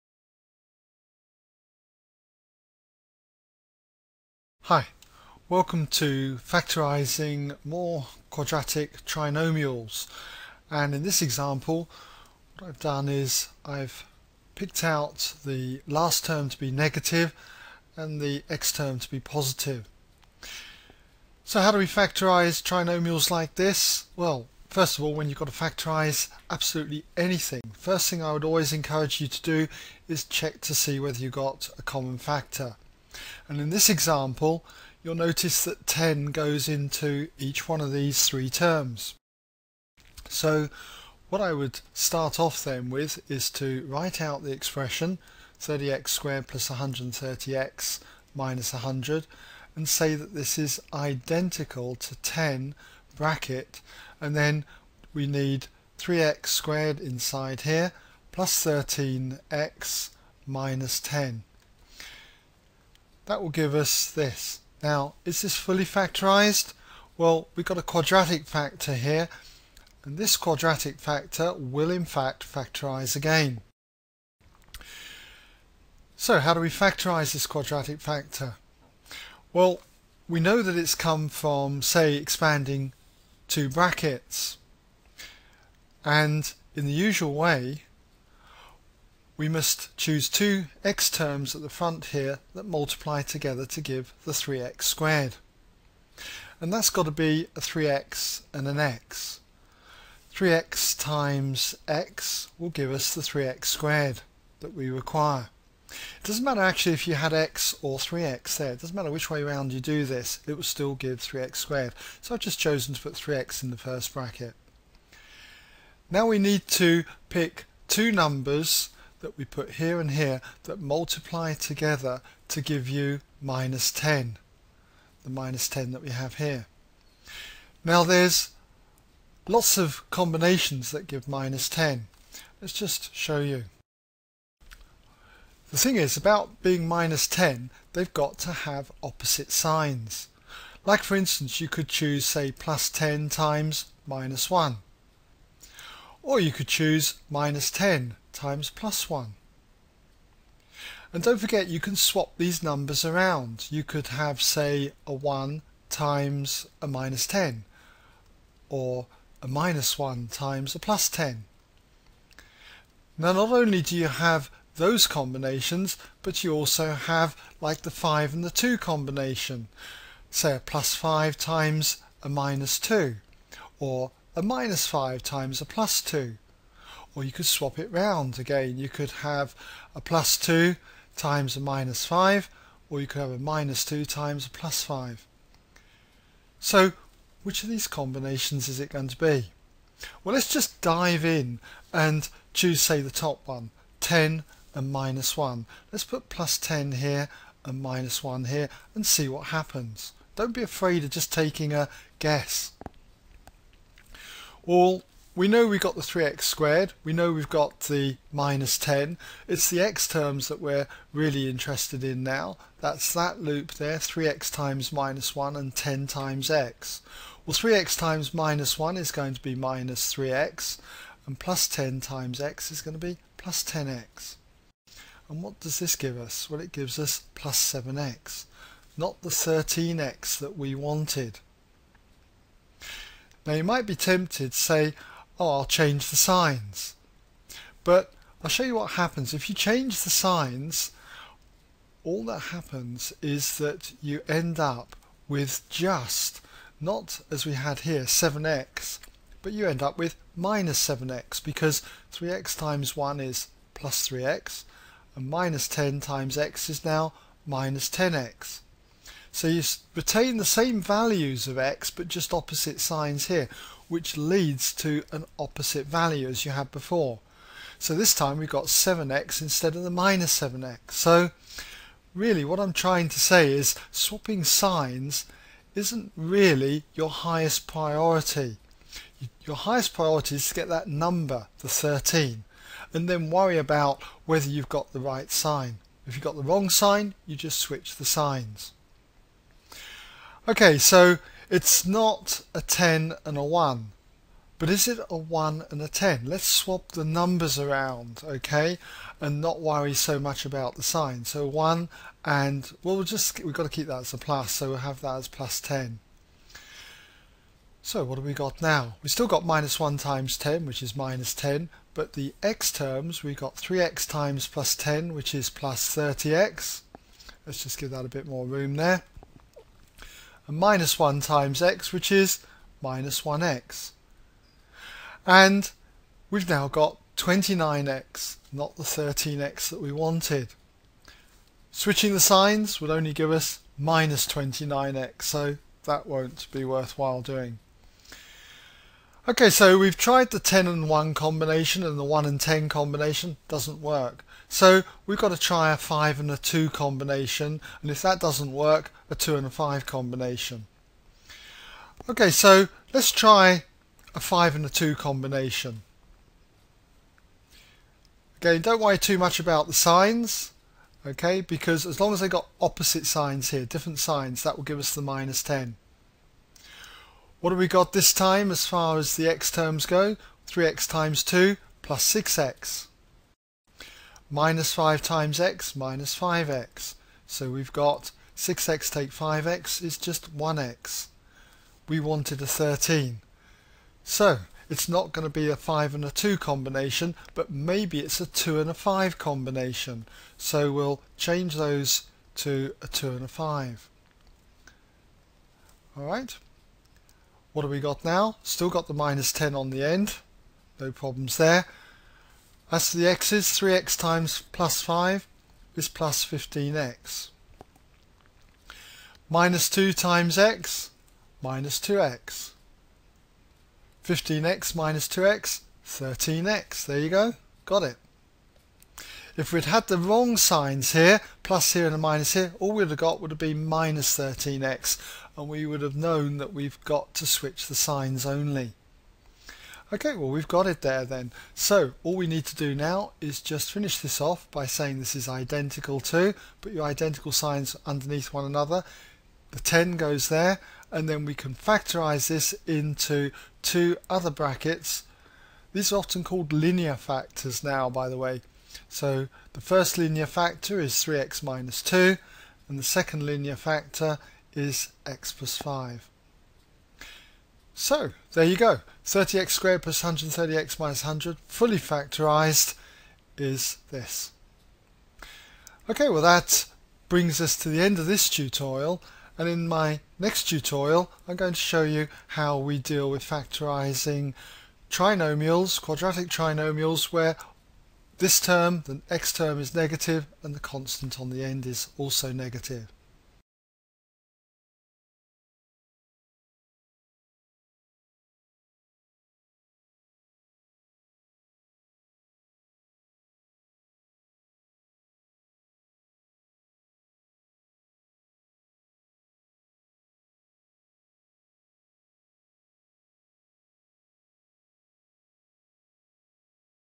Factorising Quadratic Trinomials by Inspection (4/7)
In factoring this quadratic trinomial it involves a highest common factor and the last term is negative.